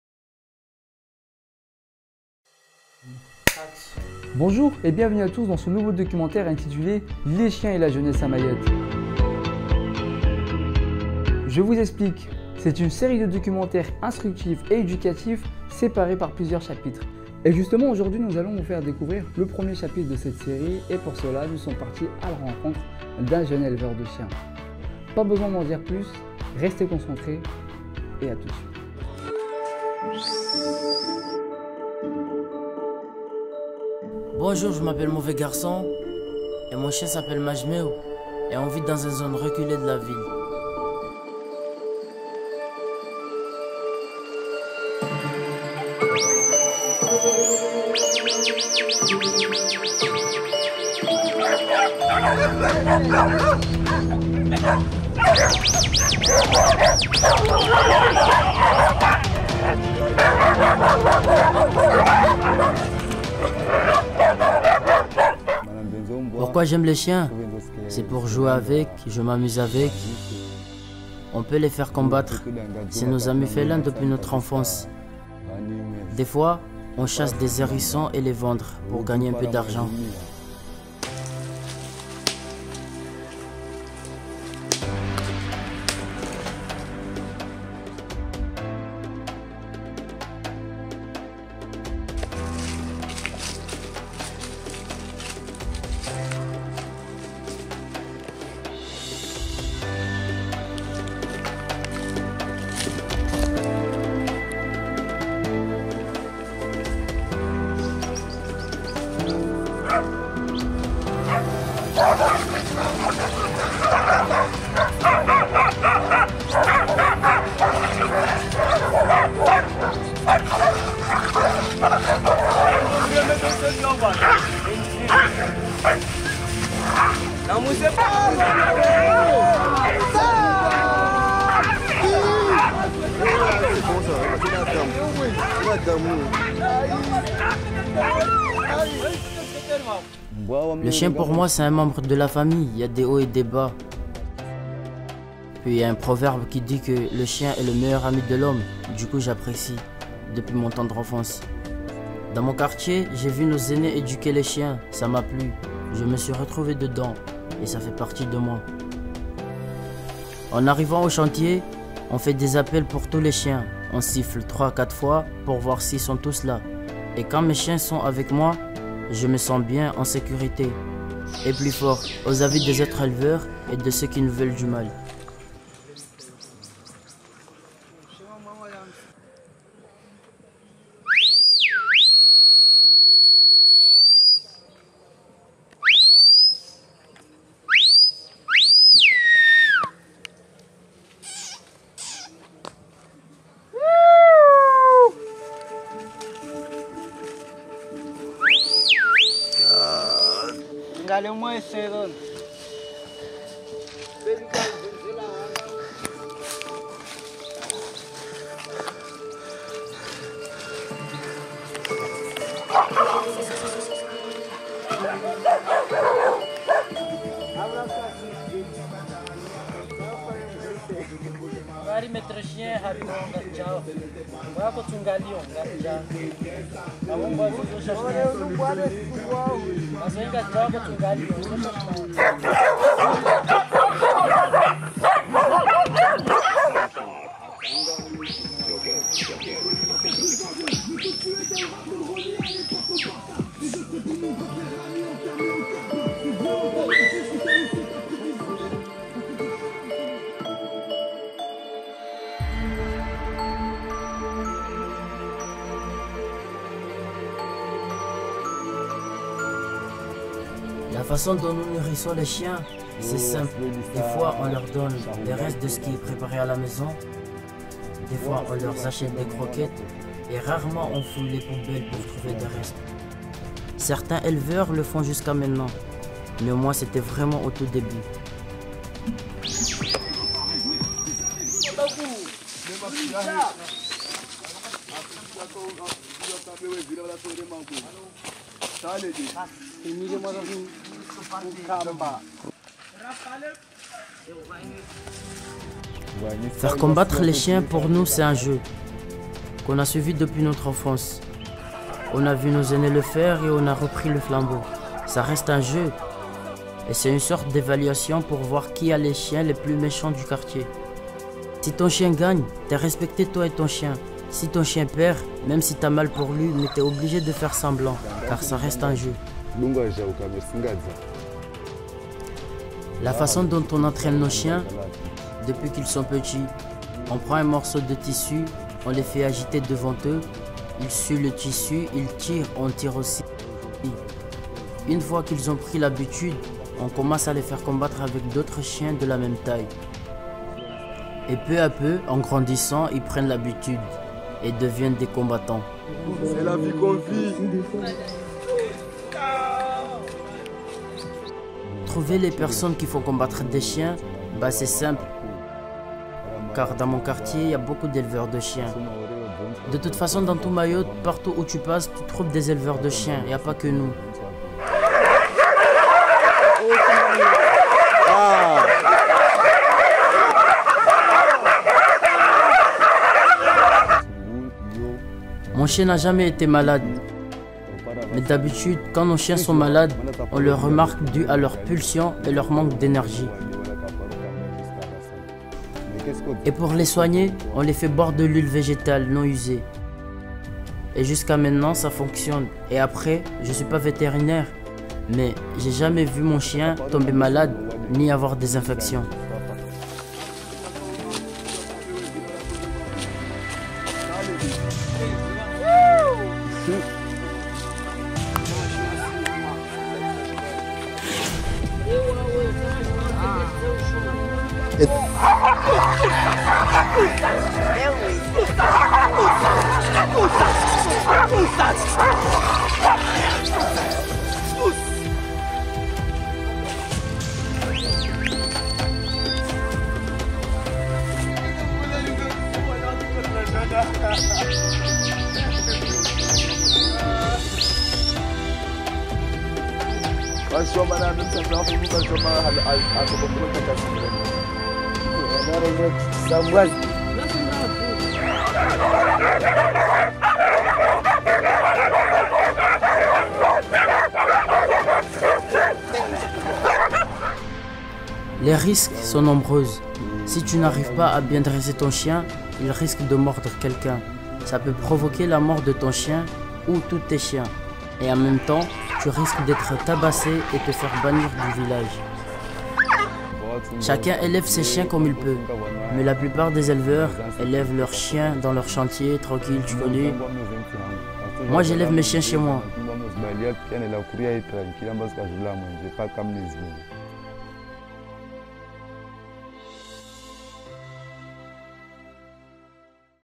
Les chiens et la jeunesse à Mayotte - Prise / Episode 1
Ampire production présente
PRISE est un projet de 8 mini -Documentaires sur Mayotte.

Chapitre I : les chiens & La Jeunesse à Mayotte

Cette vidéo a pour but de sensibiliser la population sur le sort laissé aux chiens sur l'ile de Mayotte.

il n'existe quasiment pas de structures d'accueil ou d'études sur les méthodologies d'élevage de chiens sur l'ile. les jeunes s'abandonnent (s'aventure) à une élevage illégal, souvent transmis (inspiré de) par leurs grands frères ou par théorie (imaginaires).

Cette vidéo est une alerte, une demande d'aide des politiques sur la situation.
Des centres d’éducation et où de dressage ses chiens peuvent être une solution…

Objectifs de la vidéo :
- sensibiliser les politiques, les jeunes et la population sur le sort des chiens à Mayotte.

- Video à caractère éducatif !
- alerter les structures adaptées pour la prise en charge des chiens .

Merci
Film écrit par AMPIRE PRODUCTION, Réalisé par MYSTER  Mariox
Co producteur NEEGAZ - Ingénieur son BRAYAN
Voix Off Studio AMPIRE
Financement du projet : 1 euros

➡️  La Ampire production, suis-nous ! 🙏